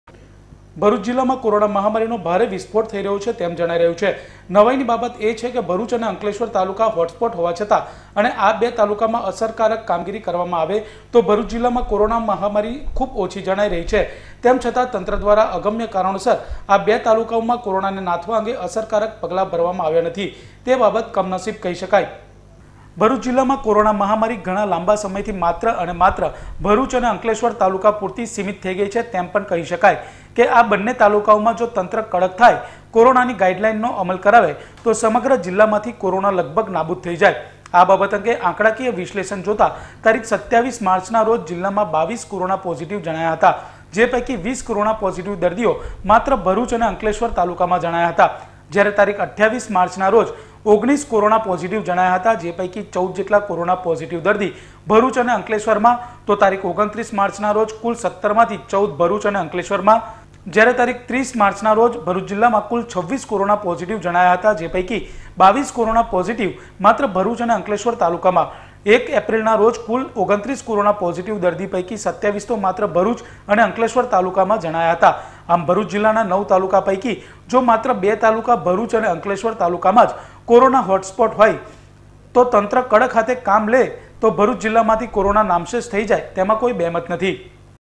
narmada samachar
ભરૂચ જિલ્લામાં કોરોના મહામારી માટે માત્ર અને માત્ર બેજ હોટસ્પોટ ભરૂચ અને અંકલેશ્વર...હાલ કોરોના વિસ્ફોટમાં 95% કરતાં વધુ કેસો માત્ર ભરૂચ અને અંકલેશ્વરમાં હોવા છતાં તત્ર દ્વારા અસરકારક પગલા કેમ લેવાતા નથી.